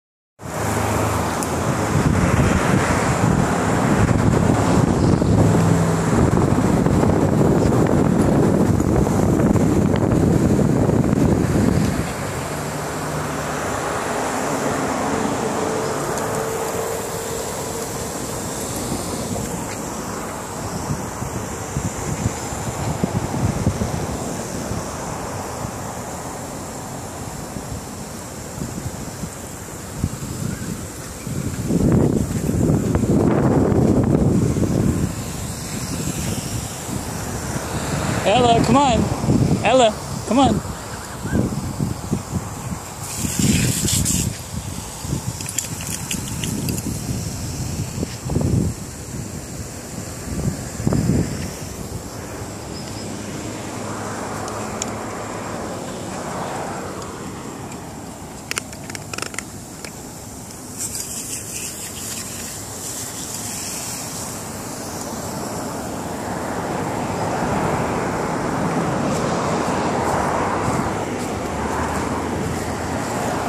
Border Collie / Flat-coated Retriever mix dog walking, playing with sprinklers
Enjoying sprinklers on a hot Florida summer afternoon (6/17/15).